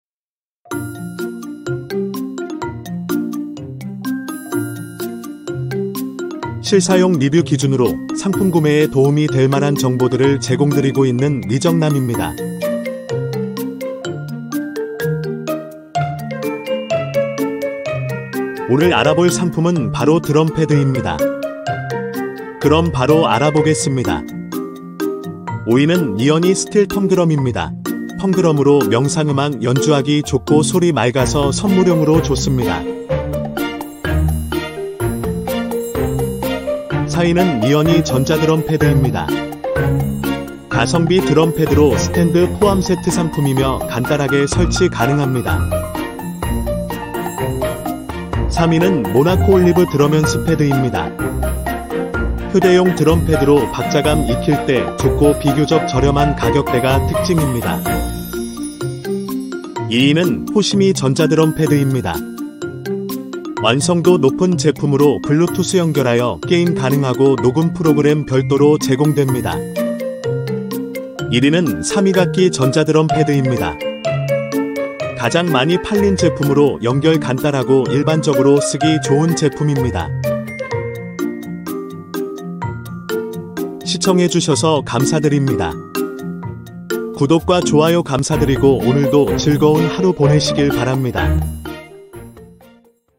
드럼패드 추천 2023 Best 5 (리뷰 간단 정리 영상)
0:25
5위
Neirny 스틸 텅드럼 (6만 1천원)

0:37
4위
Neirny 전자 드럼 패드 (10만 6천원)

0:48
3위
모나코올리브 드럼연습패드 (1만 6천원)

0:59
2위
호시미 전자드럼 패드 (15만 3천원)

1:11
1위
삼익악기 전자드럼패드 (5만 2천원)